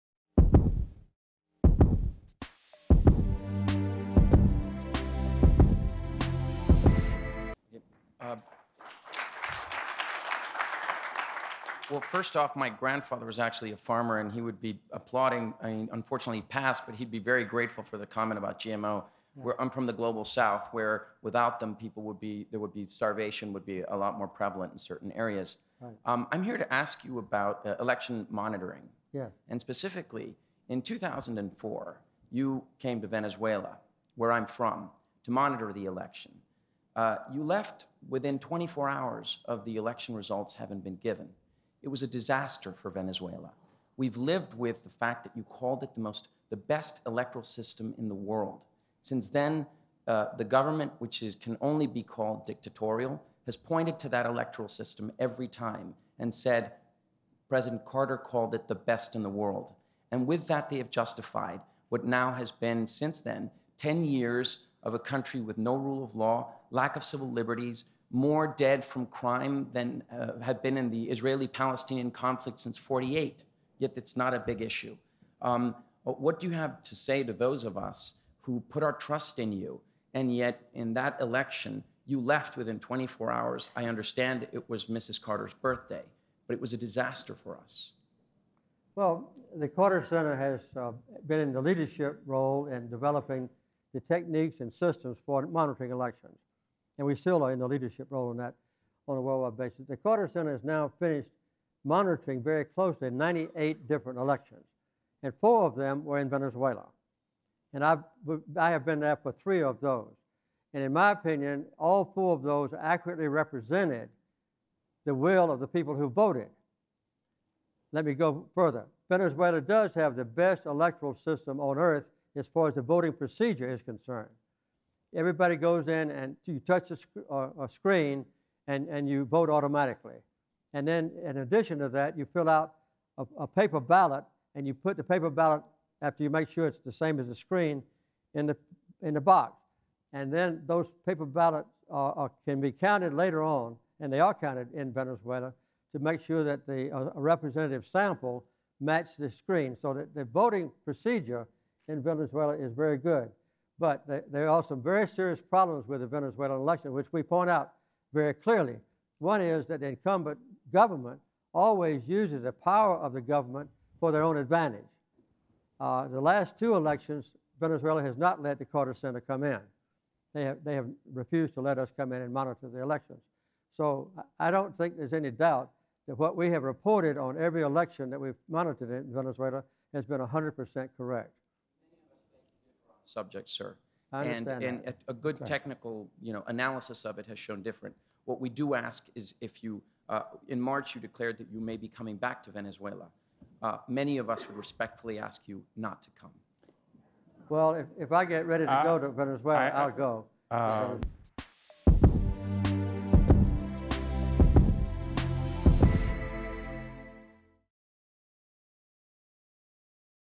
Thor Halvorssen cuestiona al expresidente Jimmy Carter sobre elecciones en Venezuela
El presidente de Human Rights Foundation (HRF), Thor Halvorssen, cuestionó al expresidente de Estados Unidos, Jimmy Carter, sobre las declaraciones que este dio el 11 de septiembre de 2012, cuando afirmó que Venezuela tiene “el mejor sistema electoral del mundo”. Las declaraciones de Carter sobre los aspectos estrictamente técnicos del proceso de votación el día de la elección, se contrastan con la erosión democrática, el abuso gubernamental y el monopolio de los medios de comunicación por parte del gobierno venezolano, que en la práctica han impedido la celebración de elecciones libres y justas durante la última década. Los procesos electorales recientes en Venezuela se han caracterizado por: (1) la ausencia de misiones de observación electoral (sólo se han permitido misiones de “acompañamiento” electoral de UNASUR, que carecen de las facultades fiscalizadoras tradicionales de una misión de observación electoral, y que “sin formular valoraciones, da[n] fe del desarrollo” del proceso); (2) la inhabilitación de candidatos opositores por parte del Consejo Nacional Electoral; (3) el cierre de medios de comunicación independientes (Radio Caracas Televisión) o la compra de medios de comunicación por parte del gobierno a través de testaferros (Globovisión, El Universal, etc.); (4) el uso de cadenas oficiales (que todos los medios deben transmitir sin excepción) que se extienden por horas a favor del candidato oficialista; y (5) la utilización del ejército para la movilización de personas (generalmente beneficiarios de programas de ayuda gubernamental) hacia los centros de votación, así como la supervisión de la votación a favor del candidato oficial en base a listados. El intercambio tuvo lugar el 15 de septiembre de 2014, en el marco de la conferencia Zeitgeist organizada por Google.

Chapters:
00:00:00 - La importancia de los OGM en el hemisferio sur
00:00:29 - Observando las Elecciones en Venezuela
00:00:44 - "El mejor sistema electoral del mundo"
00:01:04 - El problema de la criminalidad y el respeto por los derechos individuales
00:01:24 - El papel del Centro Carter en el monitoreo electoral
00:01:48 - El reflejo de la voluntad de los votantes en Venezuela
00:02:11 - Proceso de votación en Venezuela
00:02:30 - El sistema de votación en Venezuela
00:02:51 - Negación de entrada y monitoreo en procesos electorales
00:03:08 - Petición de no visitar Venezuela

¡Únase a nosotros para promover la libertad donde corre mayor riesgo!